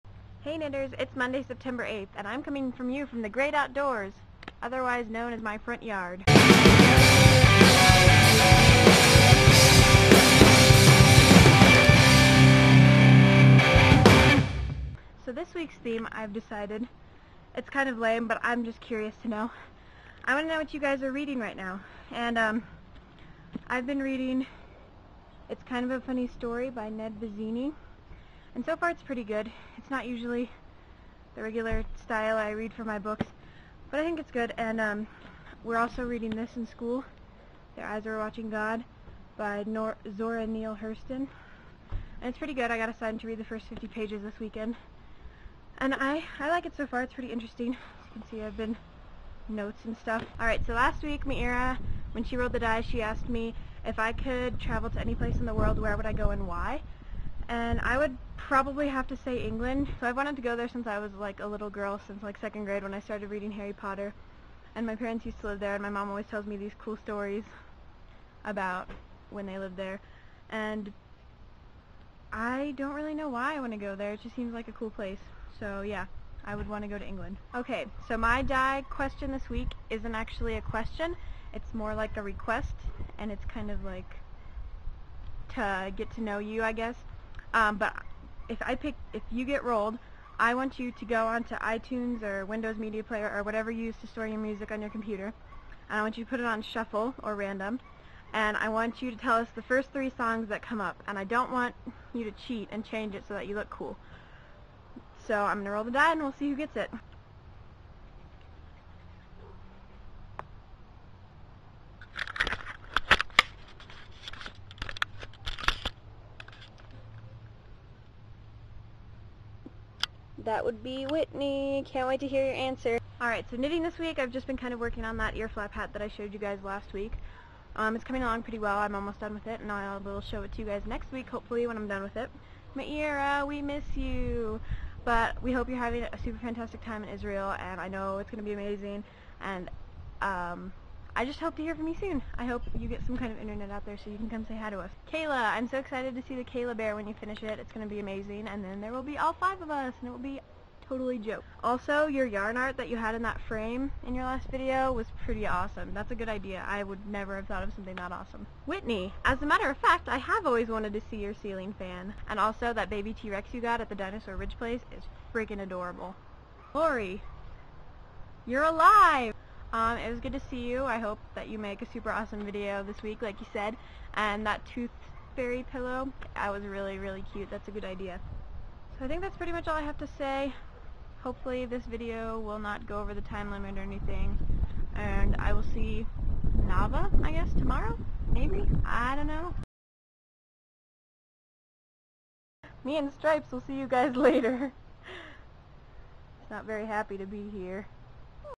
5AK 9/8: Jenny is Nature Woman... Kind Of.
jenny vlogs from the great outdoors!!  earflap hats, cats, and die, oh my!